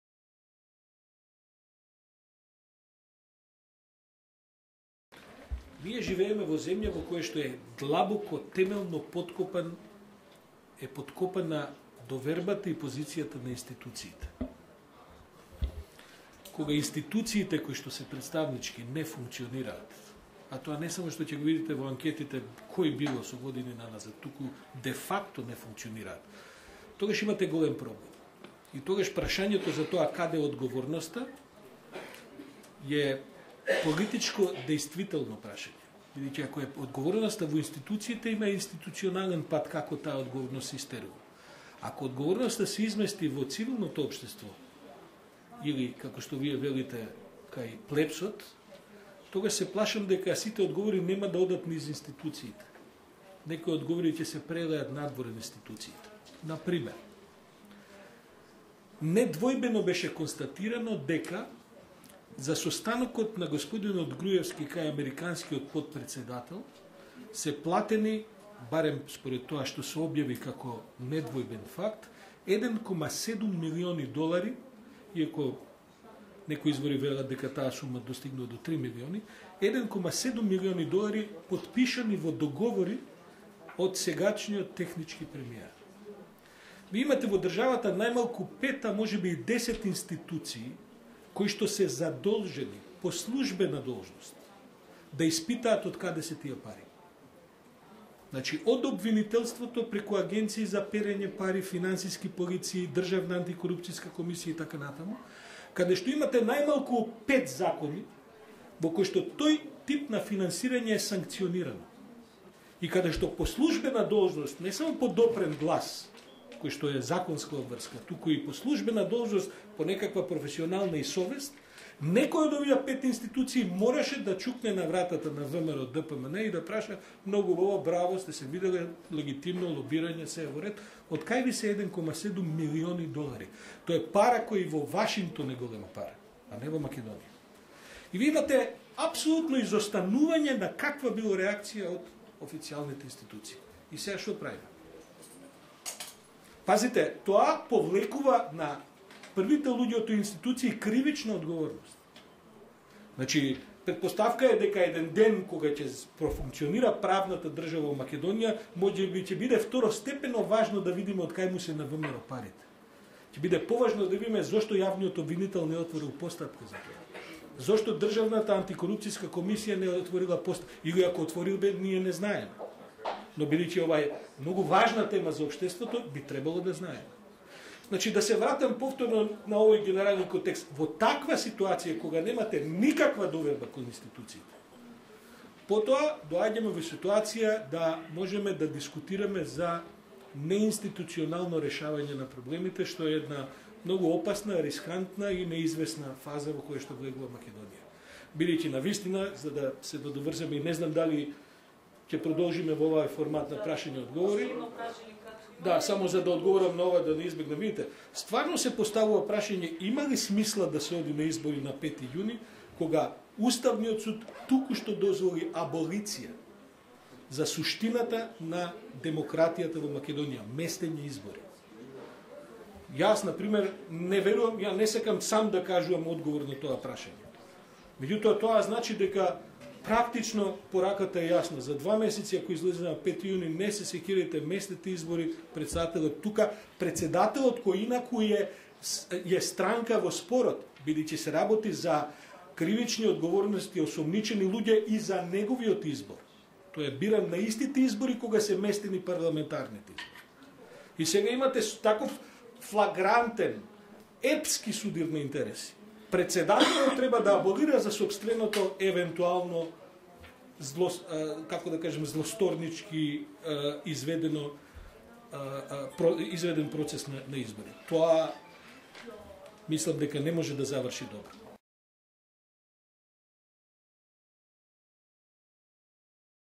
Орданоски: Груевски платил 1,7 милиони долари за средба со американскиот потпретседател
Груевски платил 1,7 милиони долари за средба со американскиот потпретседател, а институциите задолжени да спречуваат коруптивни активности не реагираат. Тоа е една од тезите на вториот дел од излагањето на д-р Сашо Орданоски на експертската панел дискусија „Плурализмот и политичката криза“ одржана на 17 март 2016 година.

Настанот е дел од проектот Слободни избори за слободни граѓанки и граѓани, финансиран од Министерството за надворешни работи на Сојузна Република Германија.